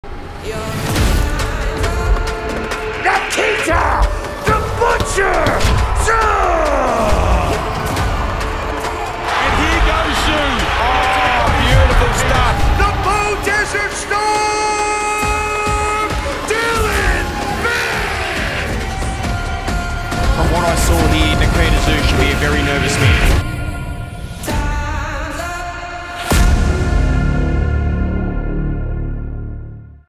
Nikita Tszyu vs Dylan Biggs | Fight Teaser
Nikita Tszyu will challenge Dylan Biggs for the prestigious Australian Super Welterweight Title on 22nd November, 2023 at Newcastle Entertainment Centre, Australia. Two KO artists going head-to-head in a career defining moment for both.